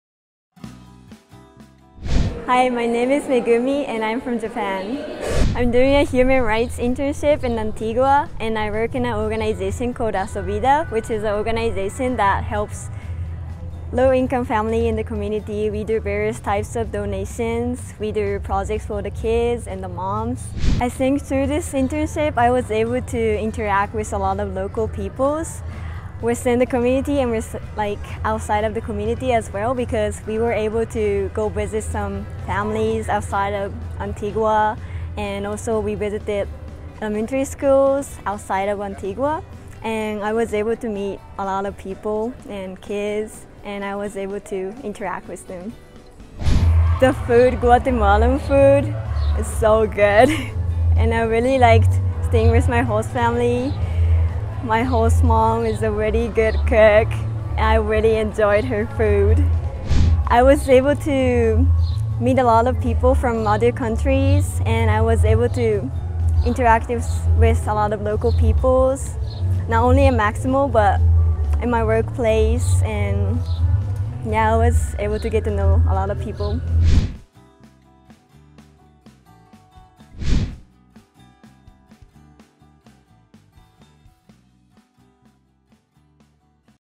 Human Rights Internship in Guatemala - Megumi
Shout out to Megumi, a passionate student from Tokyo University of Foreign Studies, who recently completed her Human Rights internship with our partner NGO at MaximoGuatemala! 💙🇬🇹 Committed to education and human rights, Megumi fully immersed herself in the local culture, uncovering the challenges communities face in Guatemala. From visiting vulnerable areas to exploring ways to combat poverty and inequality, she gained valuable insights from local activists that will undoubtedly shape her future path in international development. 👏🎓